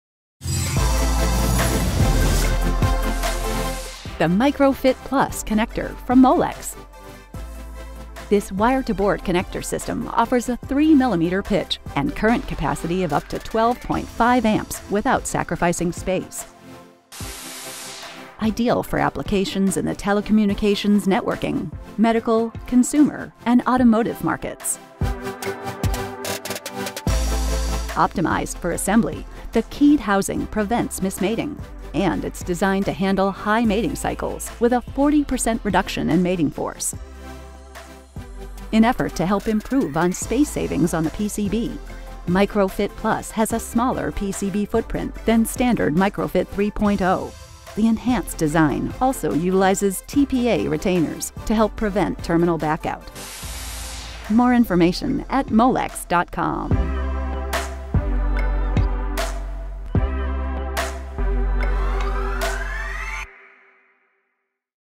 Molex - Product Spotlight - Micro-Fit+ Connector System
The Micro-Fit+ connector is a wire-to-board system which offers a 3.00mm pitch, a higher-current rating of 12.5A and superior design options for flexibility and efficient assembly.